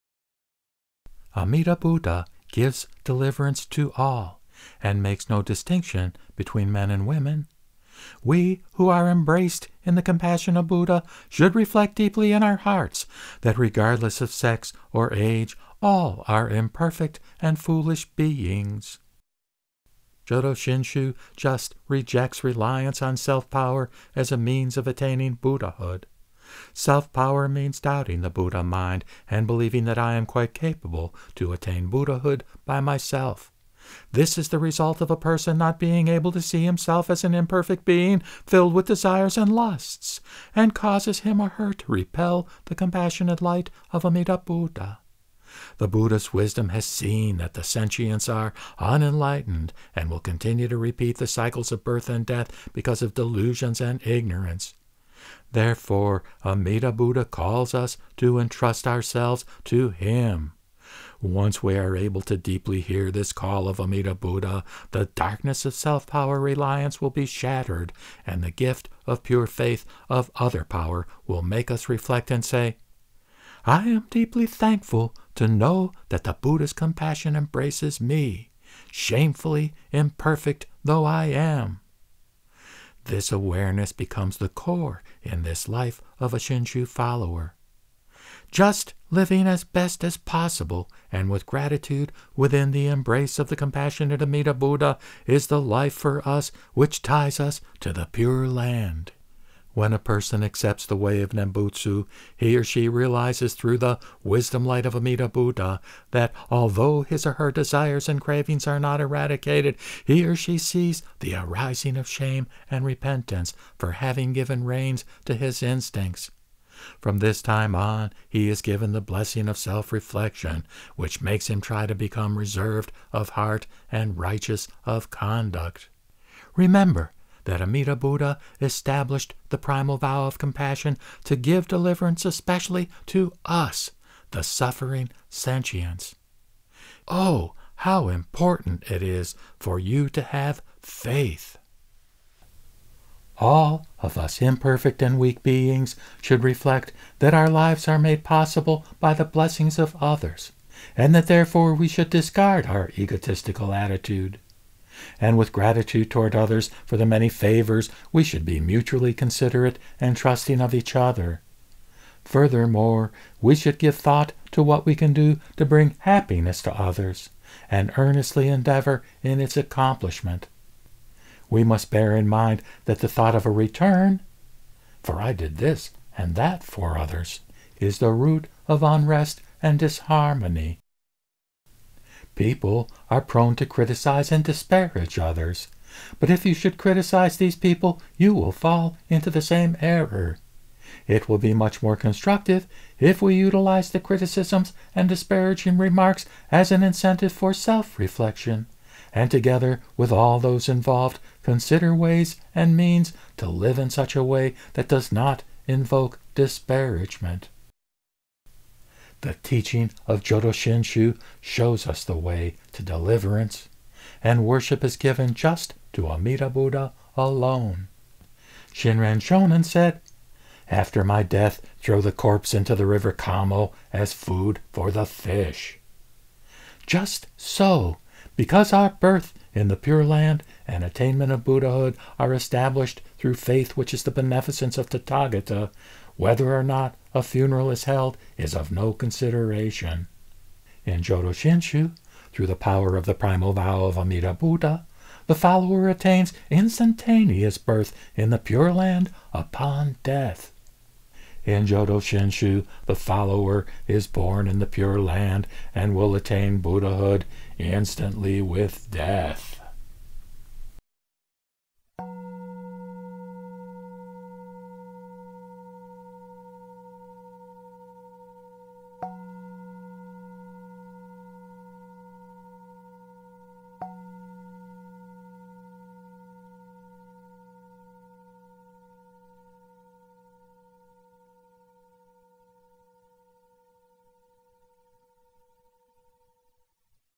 Excerpts from Jodo Shinshu Handbook for Laymen, Pt. 3
Jodo Shinshu Handbook for Laymen is a translation of Jinsei No Toi, compiled by Education Department, Jodo Shinshu Hongwanji-ha, published by Hongwanji International Center, Kyoto, Japan, 1982.

Namo Amida Butsu.